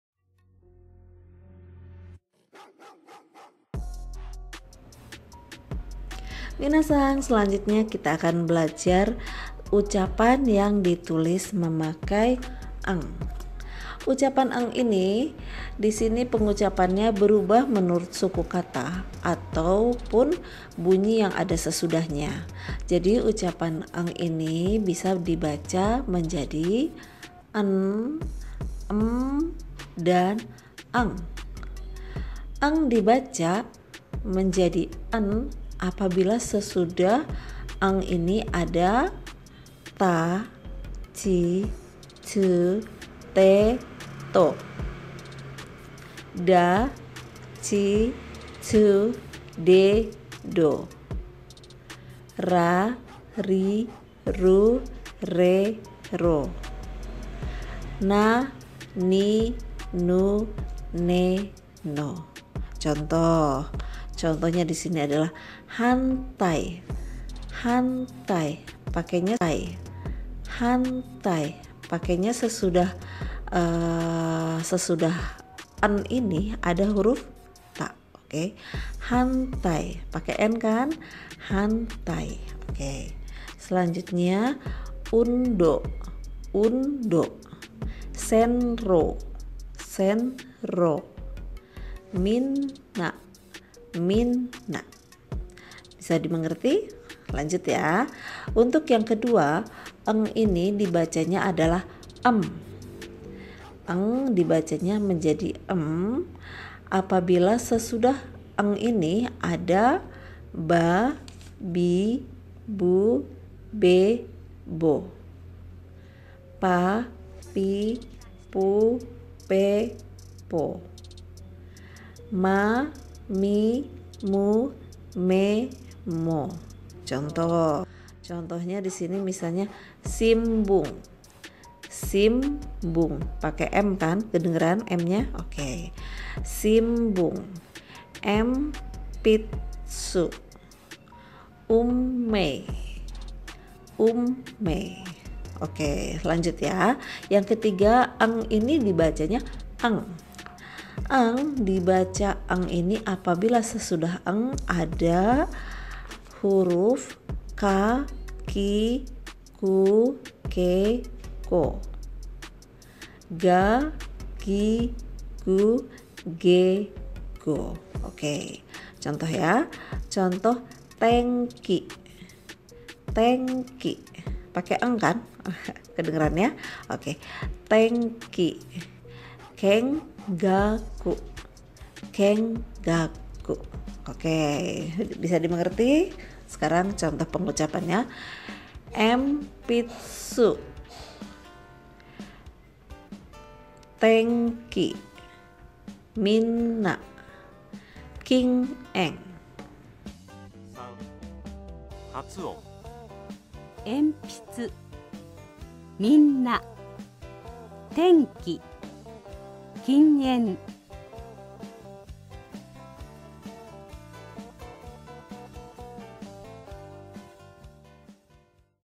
belajar bahasa jepang,kursus bahasa,mengerti bahasa jepan,berbicara bahasa jepang
sebelum masuk bab 1 minna no nihonggo, kita harus tahu dulu Pengucapan んdalam bahasa indonesia dibaca m, n dan ng